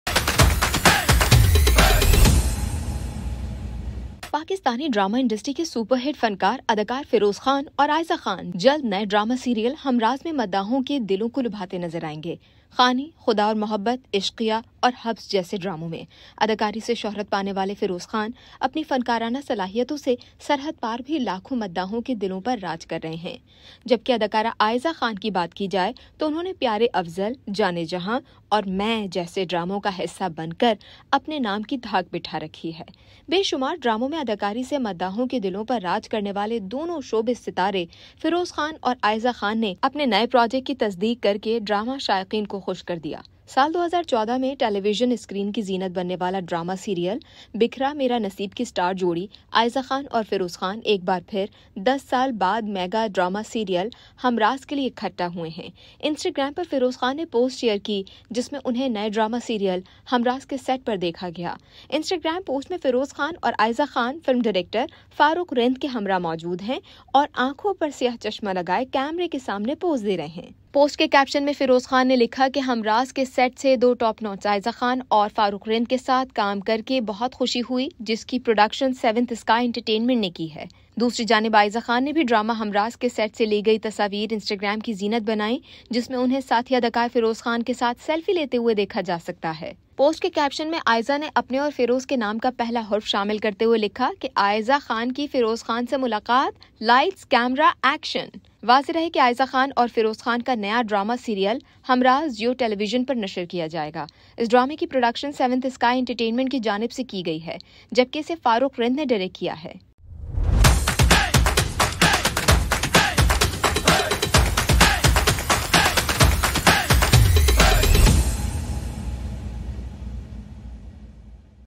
Humraaz, Feroze and Ayeza Khan Reunite in Their Most Anticipated Collaboration | Hungama Express
Fans are thrilled as Feroze Khan and Ayeza Khan come together once again for the drama 'Humraaz.' Discover why this on-screen pairing is creating so much buzz and what makes their collaboration so special.